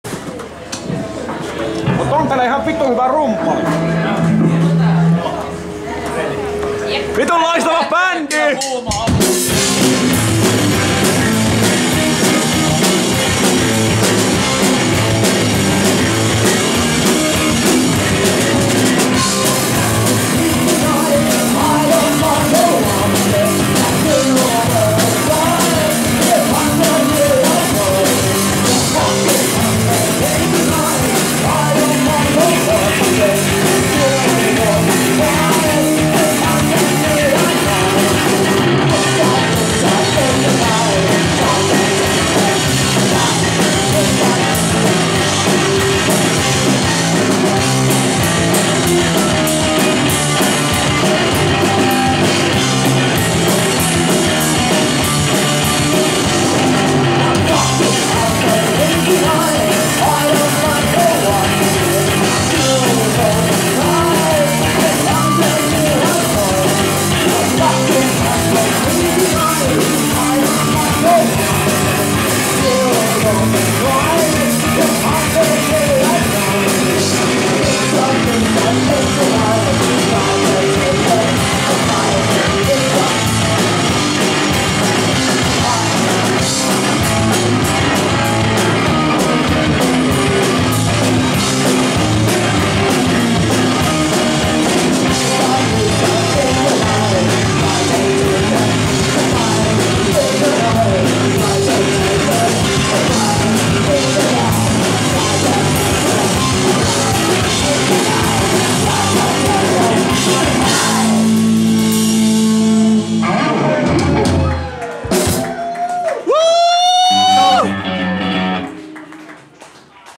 Psycho-Lorna in Punk & Roll Party Festival
Psycho-Lorna in Punk & Roll Party Festival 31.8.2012 @ Vastavirta-klubi, Tampere, Suomi - Finland.

Video shot by Krisse Chrissie Heart Sydän.